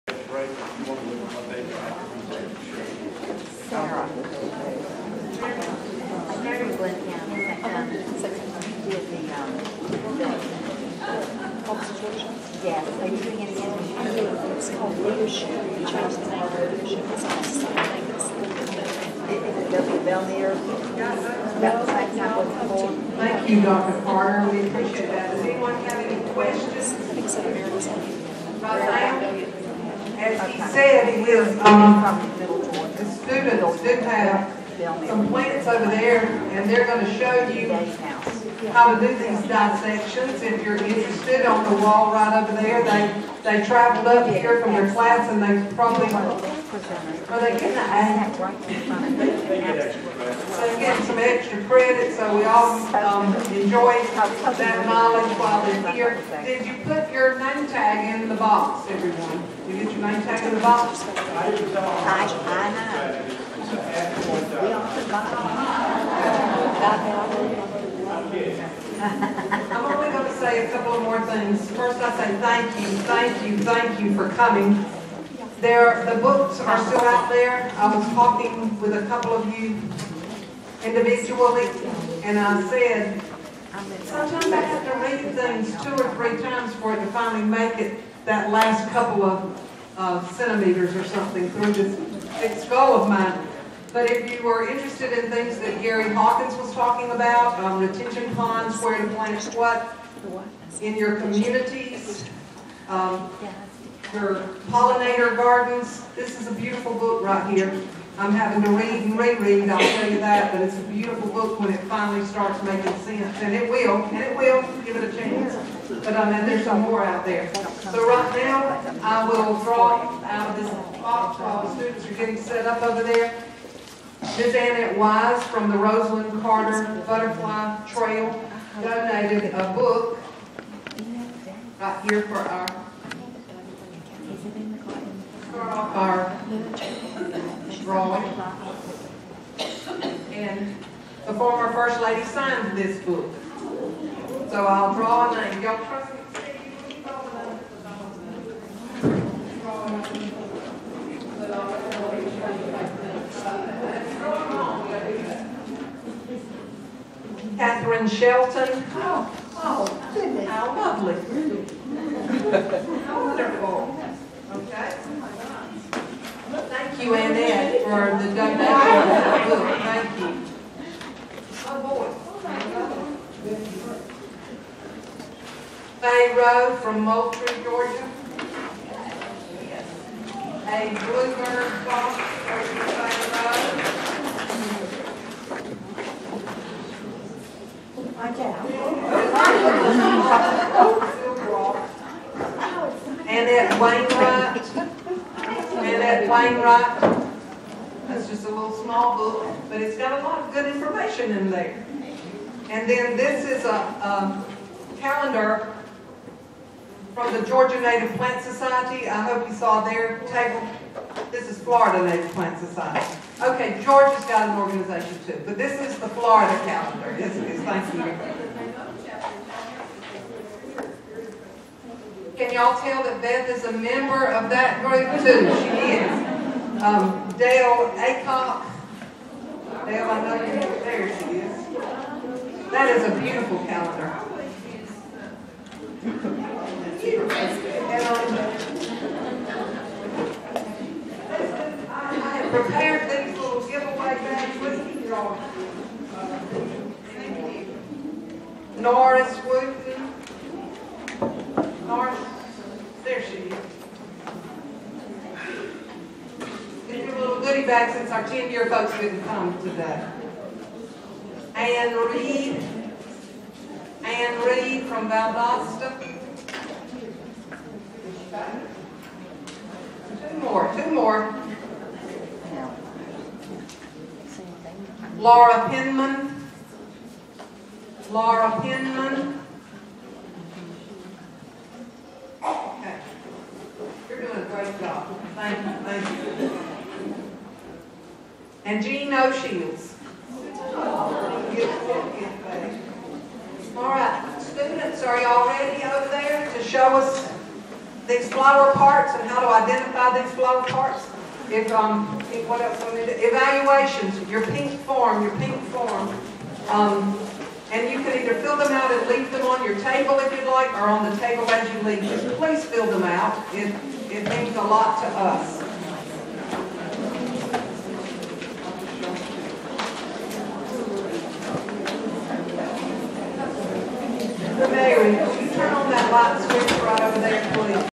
Amy Bowen Carter - Thank you and Symposium wrap up @ SGNPWFS - 2017-03-22
South Georgia Native Plant  and Wild Flower Symposium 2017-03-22